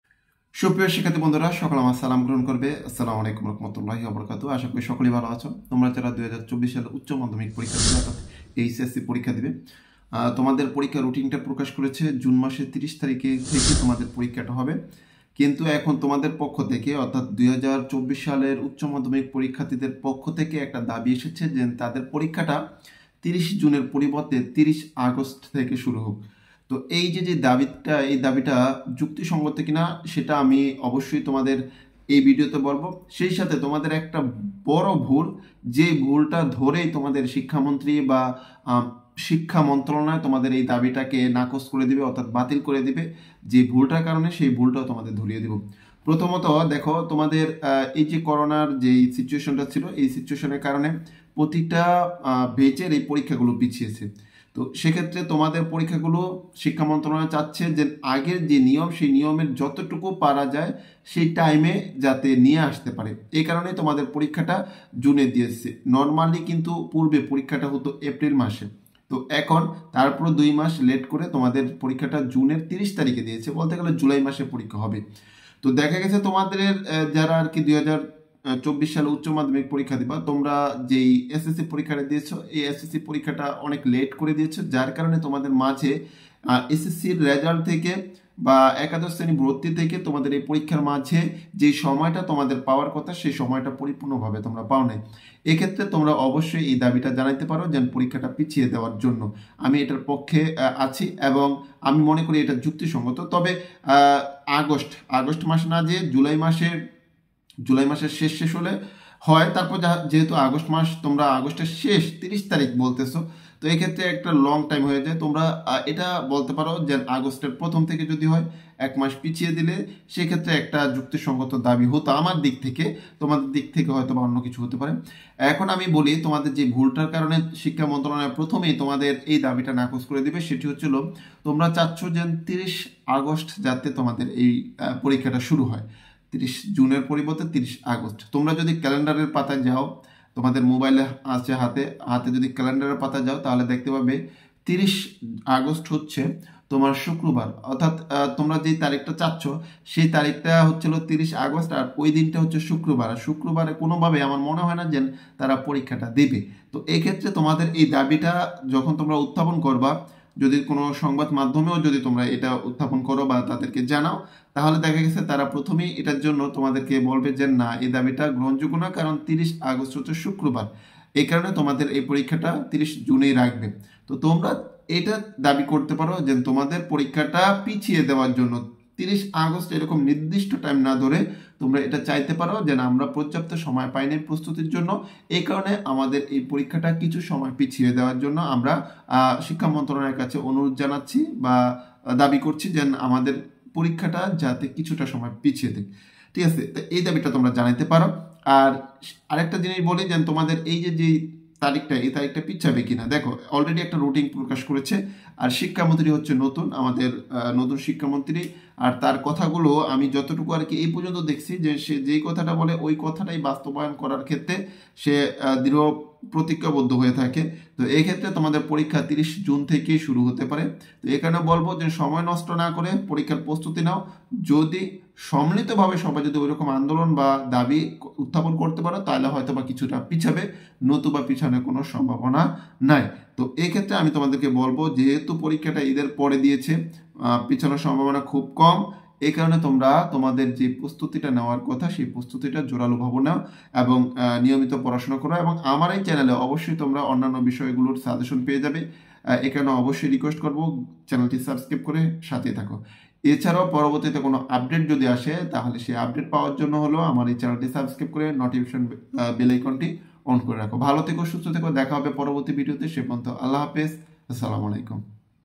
৩০ আগস্ট এইচএসসি পরীক্ষা, কতটুকু যুক্তিসঙ্গত? HSC Exam 24 date | এইচএসসি পরীক্ষা ২০২৪ এর সময়সূচি
আগামী জুন মাসের ৩০ তারিখ থেকে শুরু হবে এইচএসসি পরীক্ষা ২০২৪।  রুটিন প্রকাশ করেছে শিক্ষা মন্ত্রণালয়। কিন্তু শিক্ষার্থীদের দাবি তারা প্রস্তুতির জন্য কম সময় পেয়েছে। তাই তারা ৩০ জুনের পরিবর্তে ৩০ আগস্ট পরীক্ষা নেওয়ার দাবি জানিয়েছে।